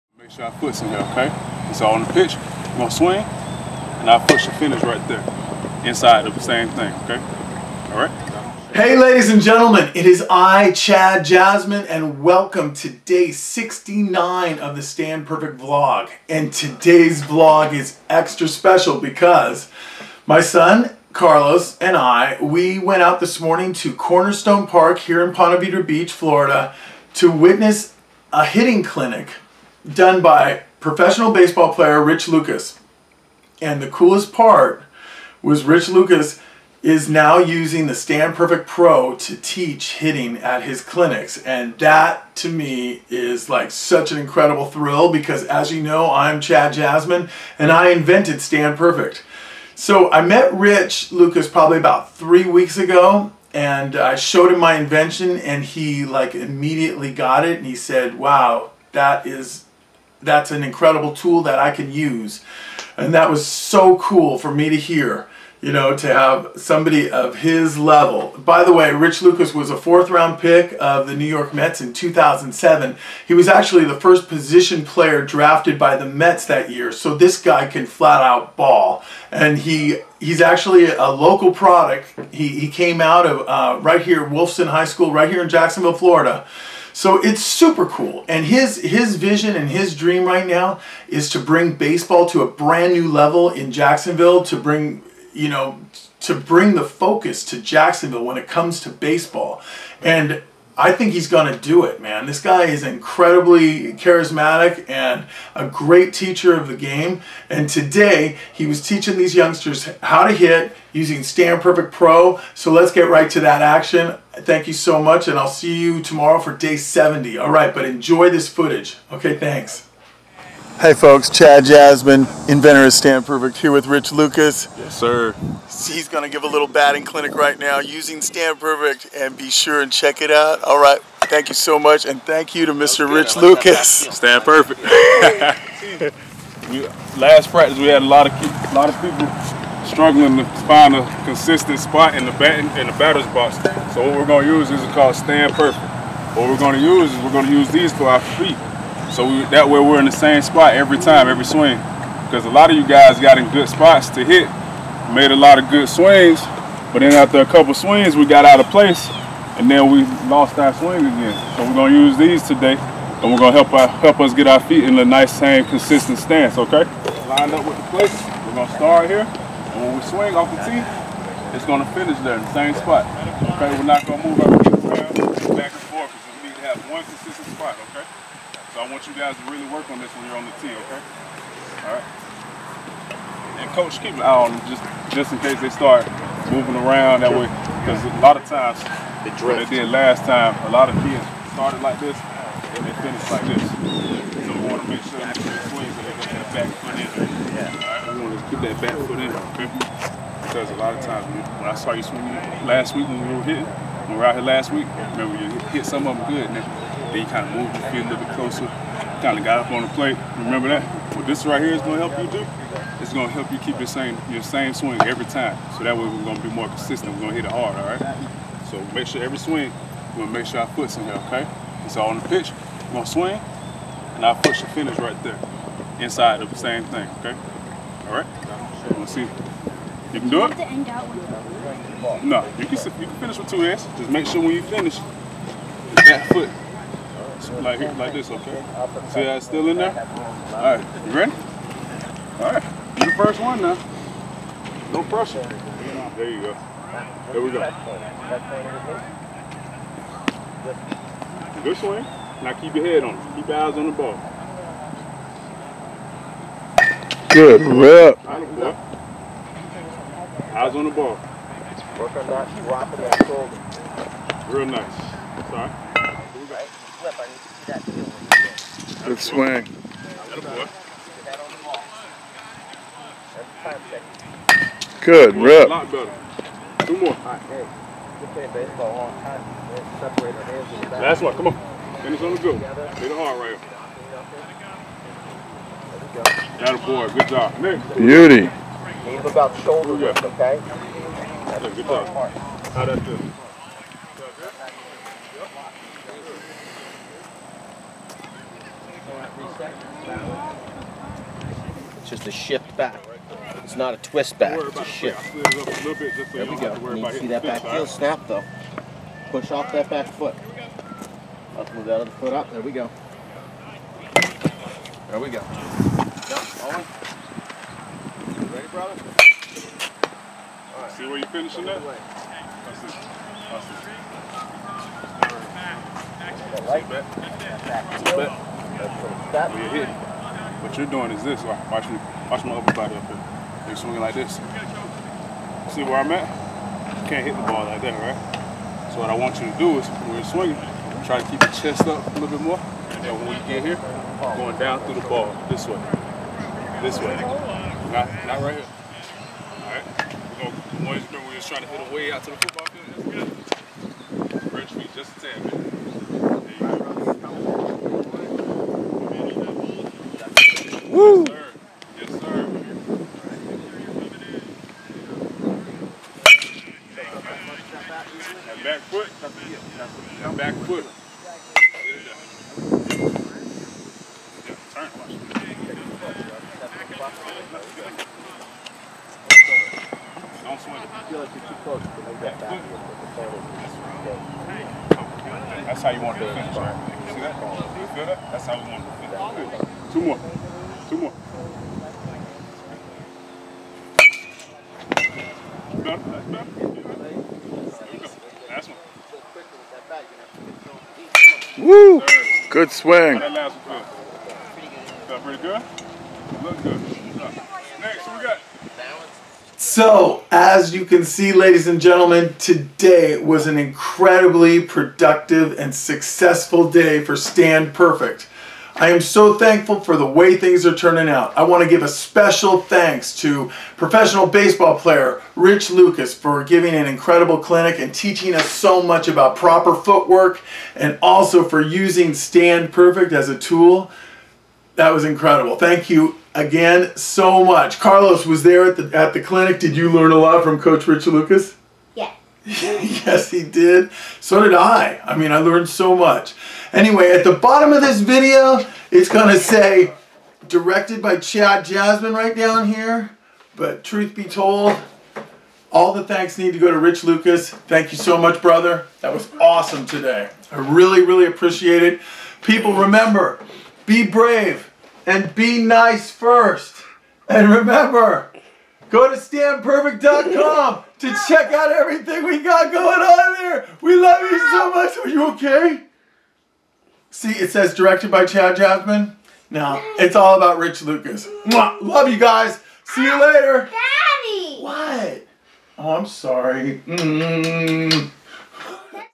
Pro Rich Lucas gives a hitting clinic
“Day 69” A Batting clinic by Pro Rich Lucas

Todays Vlog, day 69 of THE STAND PERFECT 90 DAY SUPER HUMAN BODY TRANSFORMATION CHALLENGE is the reason the Vlog was started in the first place. Yes folks, that’s right, it’s days like today that make the Stand Perfect dream a reality and for this we must thank professional ball player Mr. Richard Lucas.

In Vlog #69 Rich Lucas gives a batting clinic to some promising young Ponte Vedra Prospects and he uses the Stand Perfect Pro to do so. “I’m going to use them for every lesson I do from now on!” says the former 4th round pick of the New York Mets and former Wolfson high school star.

What?!?!?!  I MEAN HOW COOL IS THAT PEOPLE!!!

The footage in this video proves just how effective and valuable this incredible new stance trainer is! Watch Rich create improvement in this group of youngsters in a matter of minutes by using the Stand Perfect Pro to….well….shall we say… PERFECTION!!!

This is just the first in a long line of instructional videos Stand Perfect will be doing with Rich Lucas. So enjoy and look forward for a lot more to come from this guy! Oh yeah and go to standperfect.com

Thanks, Peace, Love,

-Your pals at Stand Perfect